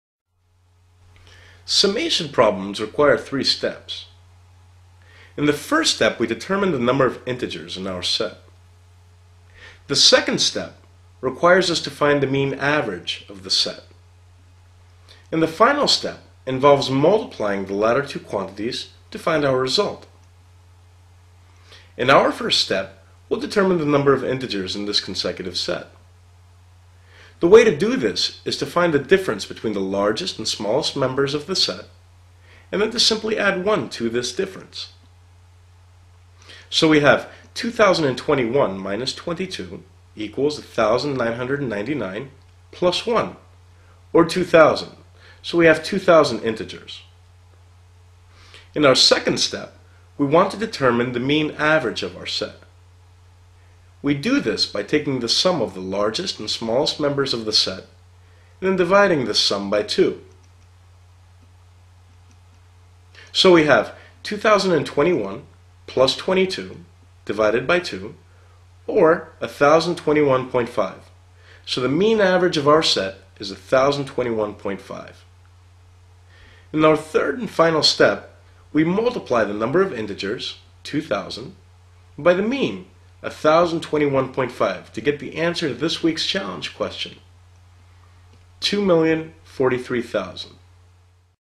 Adding Consecutive Integers (Challenging Math Question): 800score
Prof. Stergious Alexandris (perfect scores GMAT, GRE and SAT) describes a technique for adding a series of integers quickly.